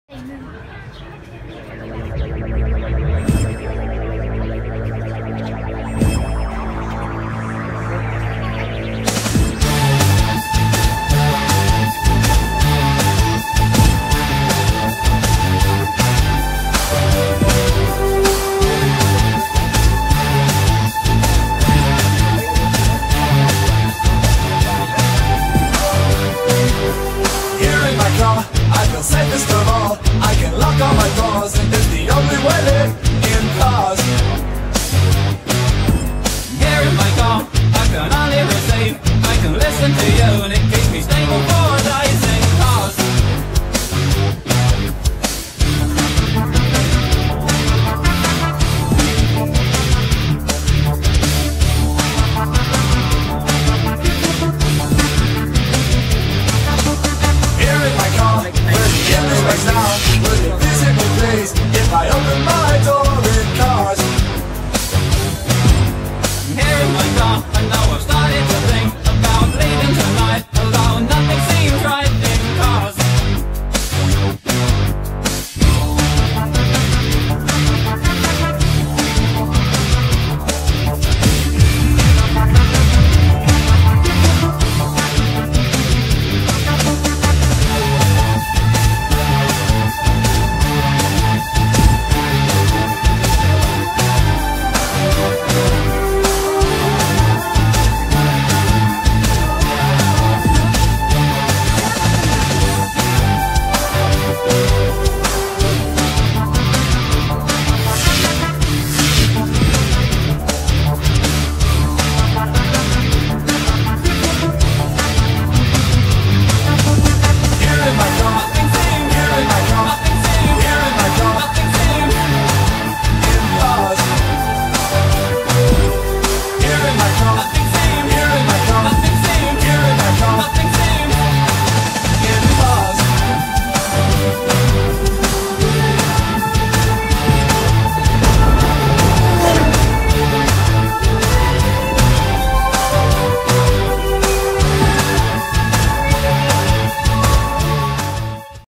Classic and Exotic Car Show
Mercedes-Benz Peterborough hosted the 2nd annual Father’s Day Cars Show in support of Peterborough Regional Health Centre (PRHC) Foundation. Classic & exotic cars were on display on Hunter street between Water and Alymer Street.
The event was well attended and we some some really cool cars.